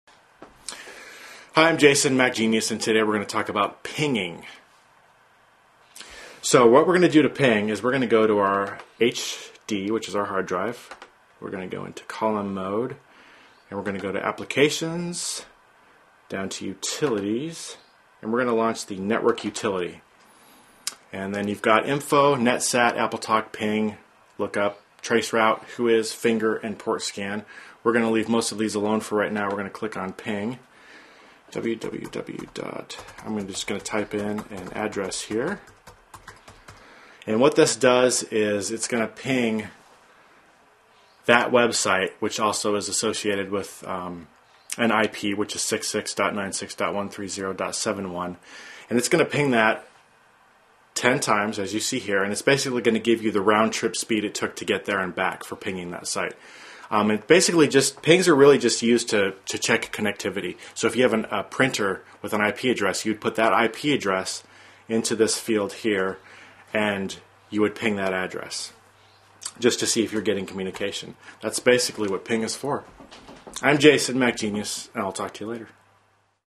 How to Ping an IP Address
How to Ping an IP Address. Part of the series: Computer Network Tips. Pinging an IP address on a computer involves clicking on the hard drive and launching network utility. Learn to ping an IP internet address with tips from a computer specialist in this free video on computer networking.